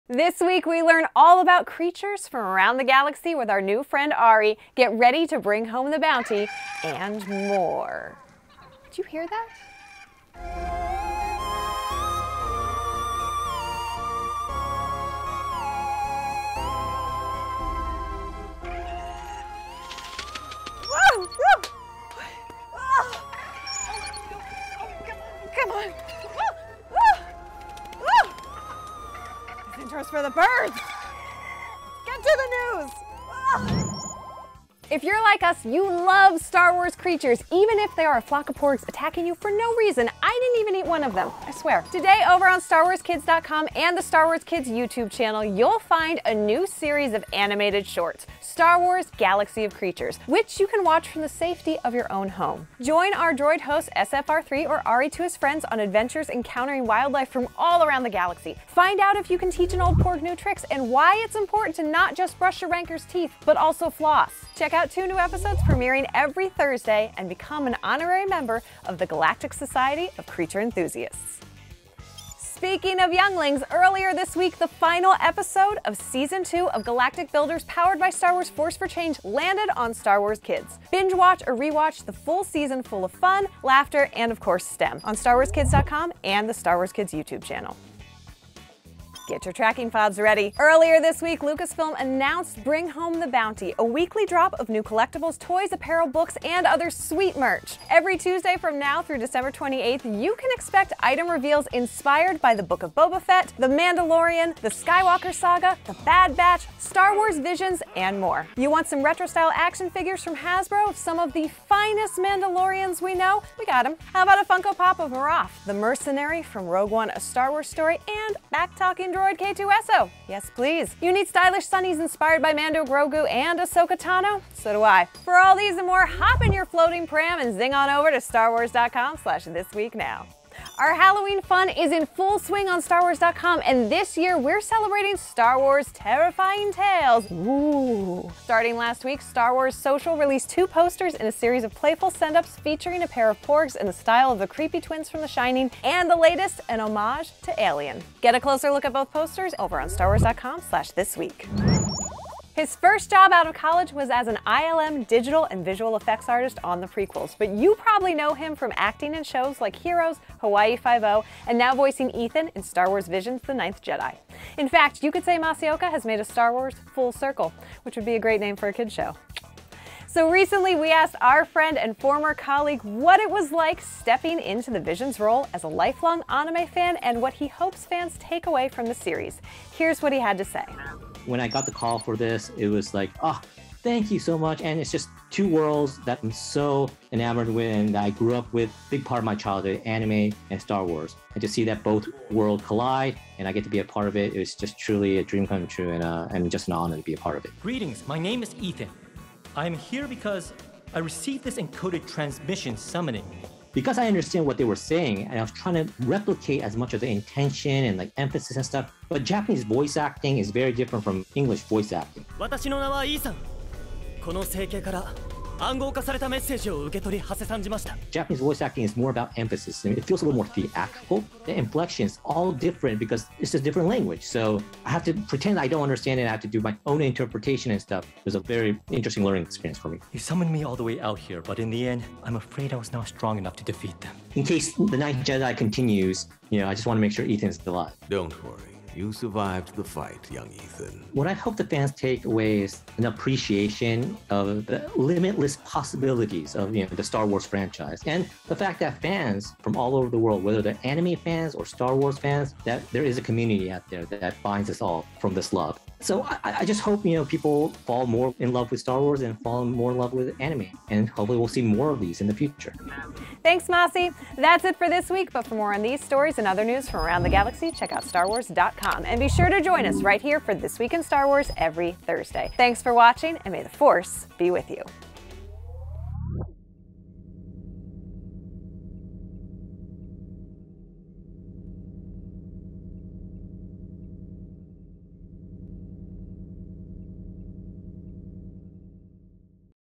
Galaxy of Creatures, Bring Home the Bounty, and More!
This week in Star Wars, we get ready to meet different creatures from around the galaxy in Star Wars Galaxy of Creatures, check out the latest batch of products for Bring Home the Bounty, and get into the Halloween spirit with some Star Wars: Terrifying Tales posters. Plus, we sit down with Masi Oka to chat about Star Wars: Visions on Disney+.